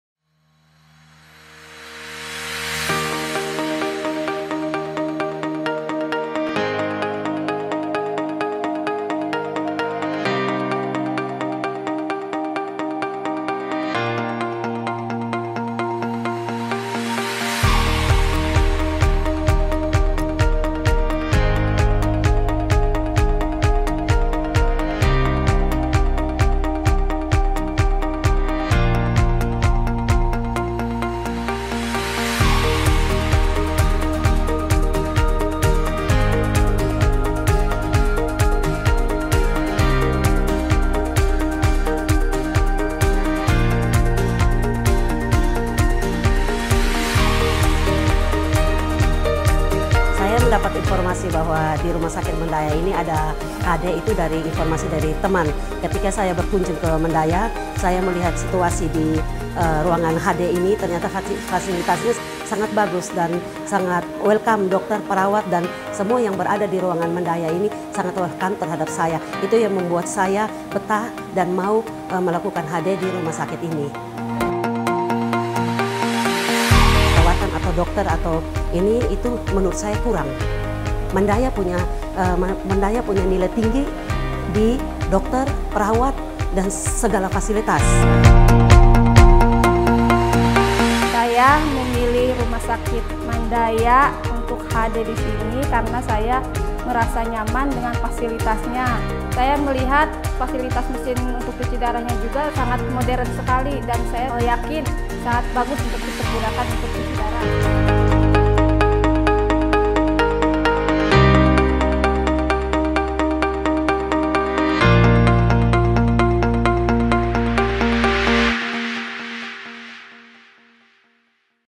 CUCI DARAH DENGAN MESIN BARU DAN MODERN DI HEMODIALYSIS CENTER MANDAYA HOSPITAL KARAWANG
Hemodialysis Center Mandaya Hospital Karawang dilengkapi dengan 23 mesin baru & modern, ruang tindakan nyaman & bersih, memiliki fasilitas ruang tunggu keluarga, fasilitas ruang modern.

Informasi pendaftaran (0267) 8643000
Jam operasional :
Senin- Jumat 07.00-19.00 wib Sabtu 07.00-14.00 wib